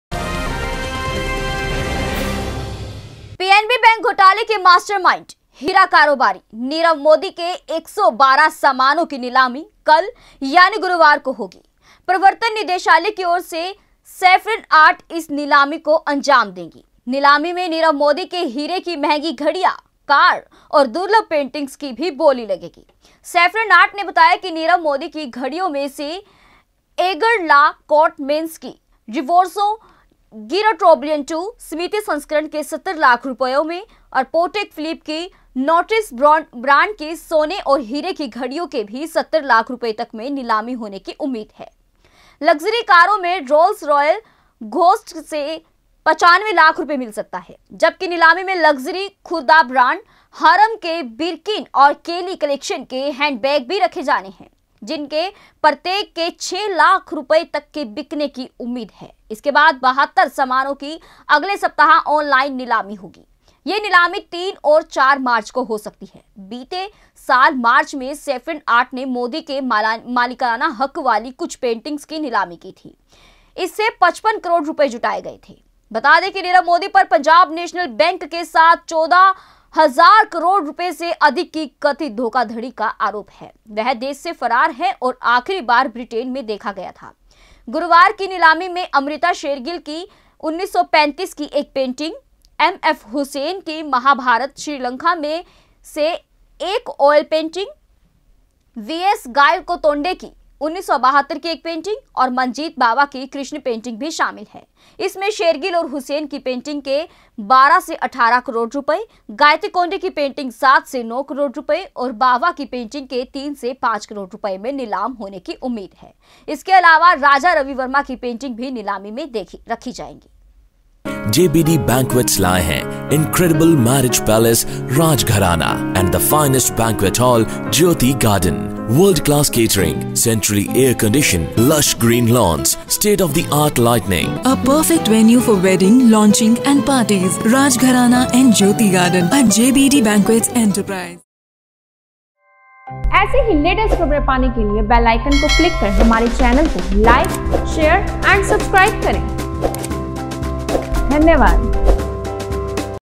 नीरव मोदी की घड़ी से लेकर कार और पेंटिंग्स तक होगी नीलाम
नीरव मोदी की घड़ी से लेकर कार और पेंटिंग्स तक होगी नीलाम, भगोड़े हीरा कारोबारी नीरव मोदी के हीरे की महंगी घडिय़ां